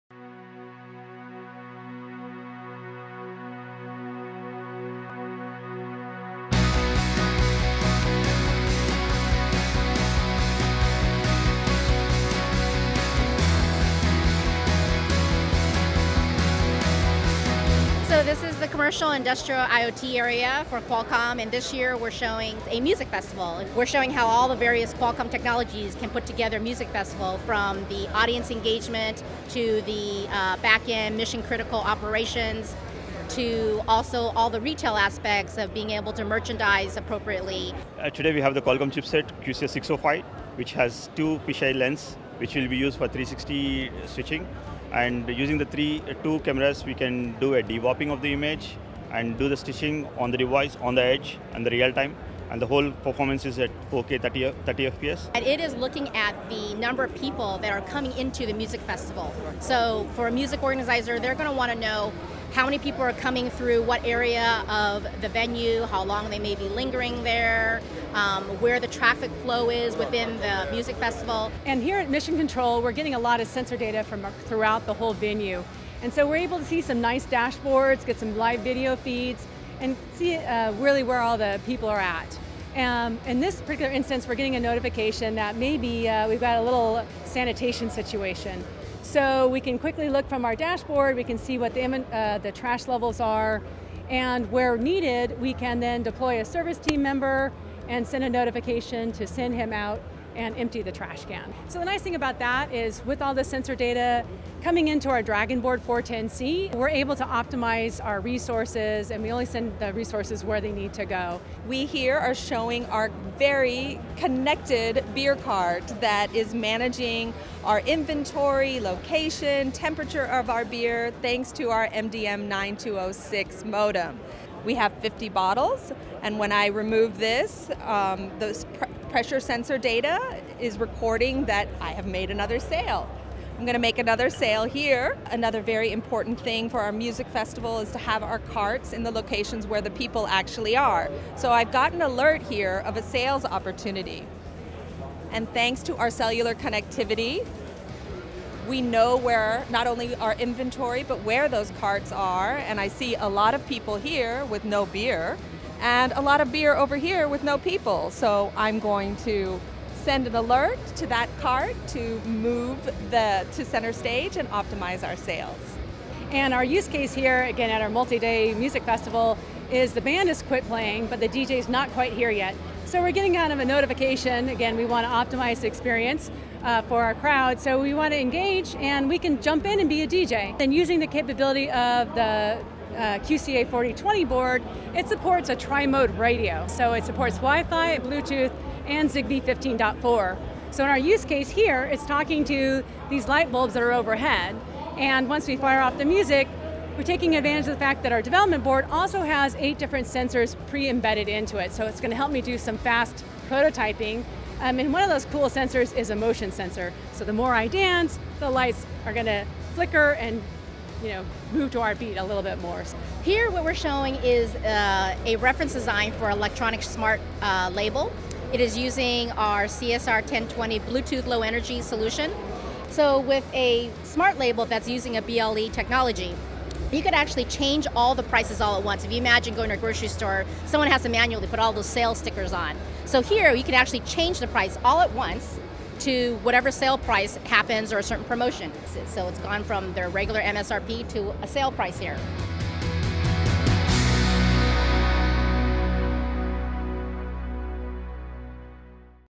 Industrial IoT Music Festival Demo
In this video, we provide some walkthrough visualizations of our music festival demo, which illustrates multiple IoT technologies and how they can all work together.